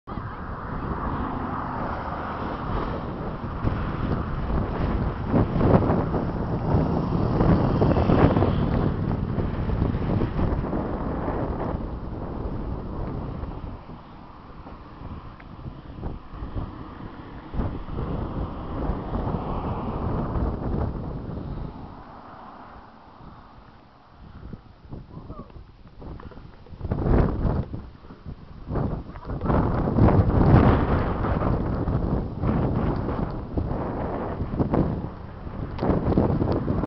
The Border Country
Standing on the hill between England and Scotland